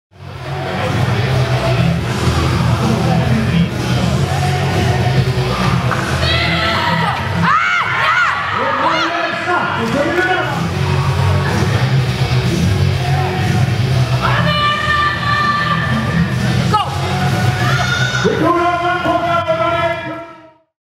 Swedish Nationals 2012 Women Heavyweight Right
Swedish Nationals 2012 Women Heavyweight Right Victoria Alveland vs Emma Edvall